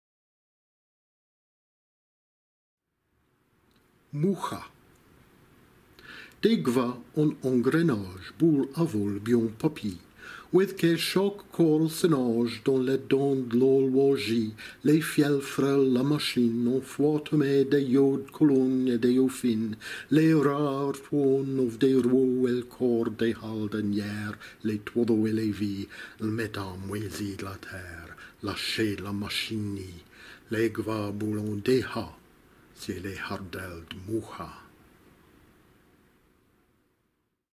Mucha
"Mucha" - eune poésie en Jèrriais par Geraint Jennings